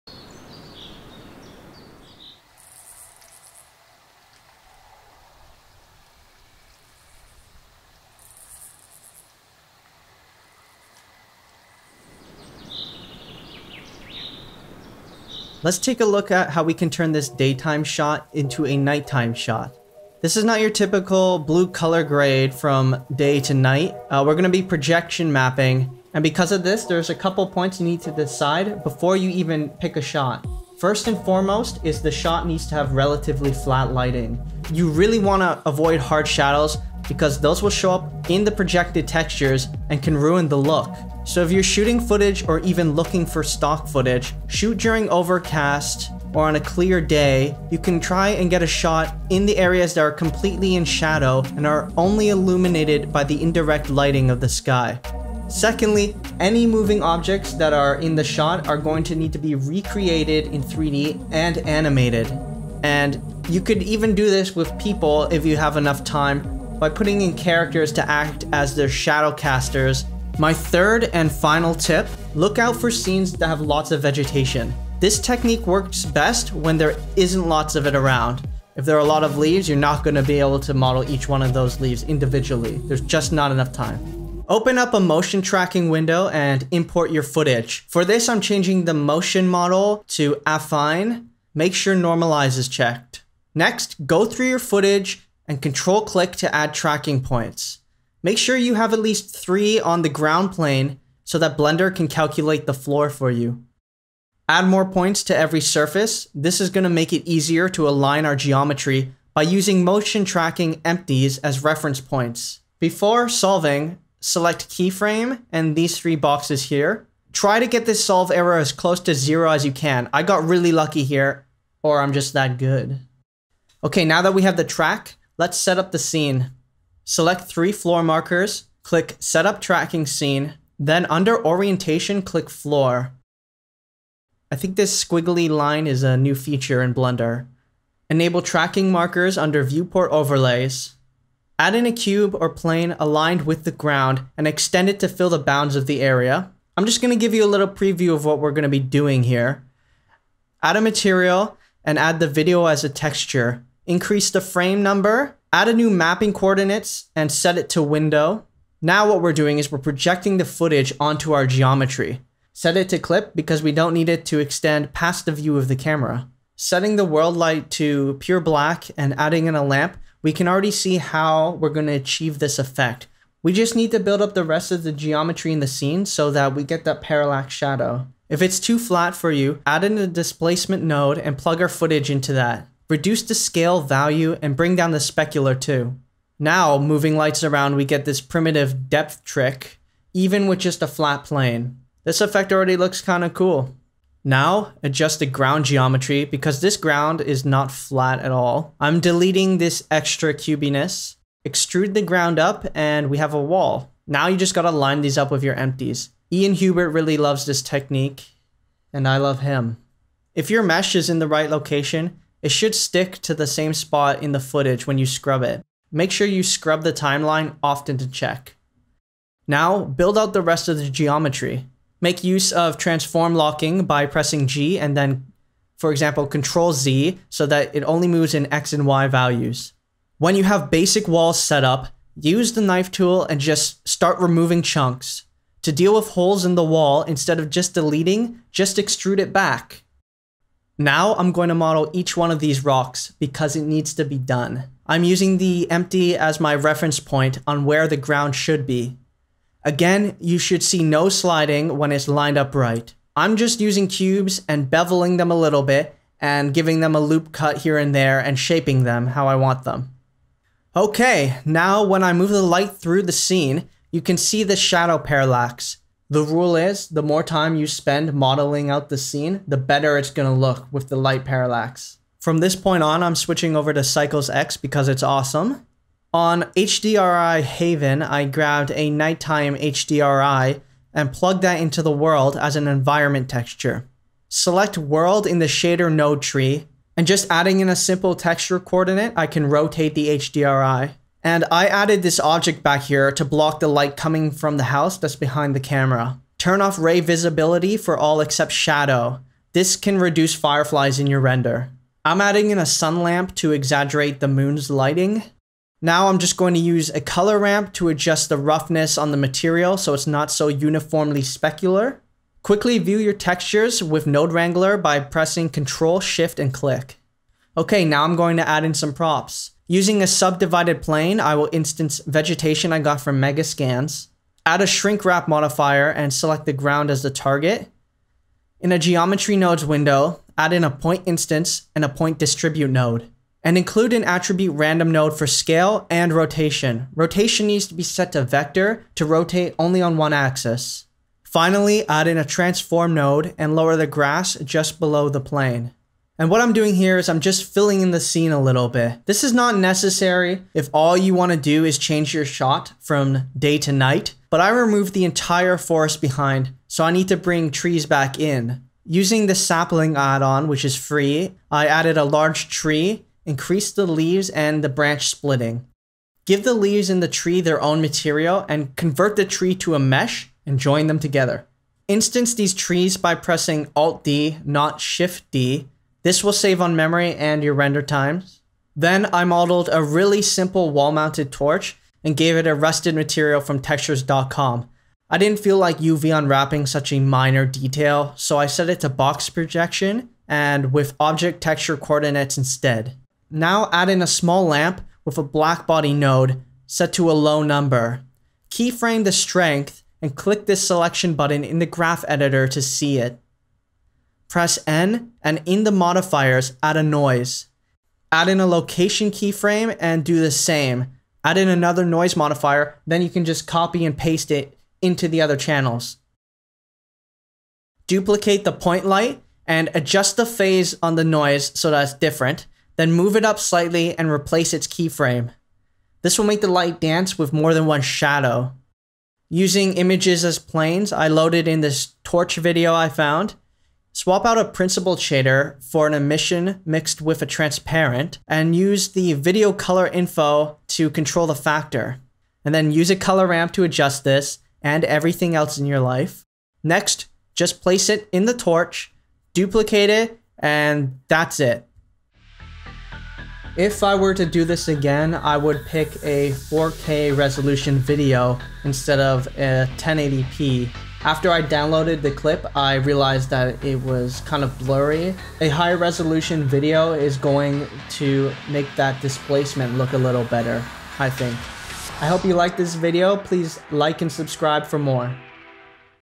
Day to Night VFX with Projection Mapping
This is my process for turning a daytime shot into a nighttime shot. This only works for particular shots.

0:00 Intro
0:15 Picking a Shot
1:24 Motion Tracking
2:00 Modelling the Scene
4:31 Lighting Setup
5:17 Adding Props
6:33 Animating the Light
7:32 Final Thoughts